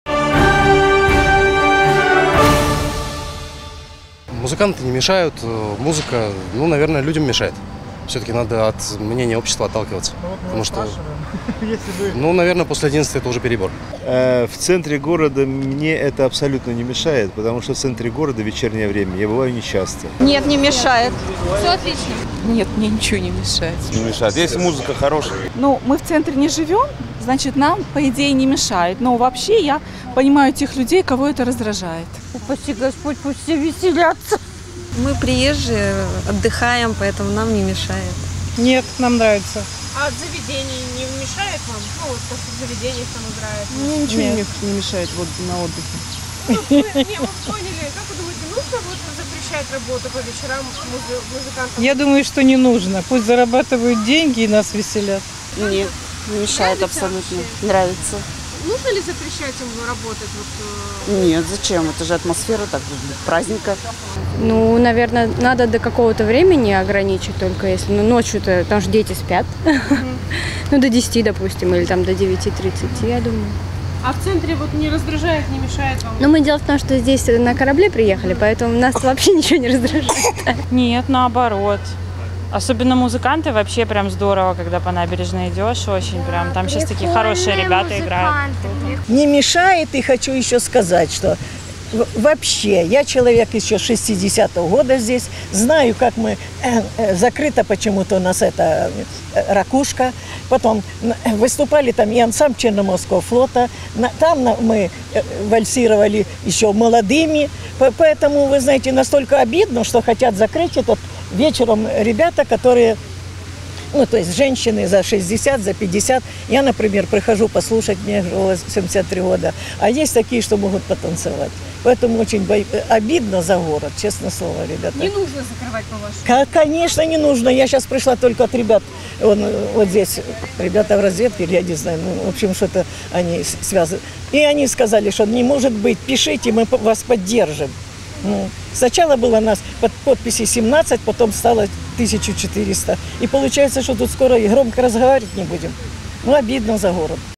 Мнение: Мешает ли вам музыка в центре Севастополя?
«Независимое телевидение Севастополя» продолжает рубрику «Мнение». Ежедневно в эфире нашего телеканала – ответы севастопольцев и гостей города на вопросы, которые интересны, актуальны и злободневны.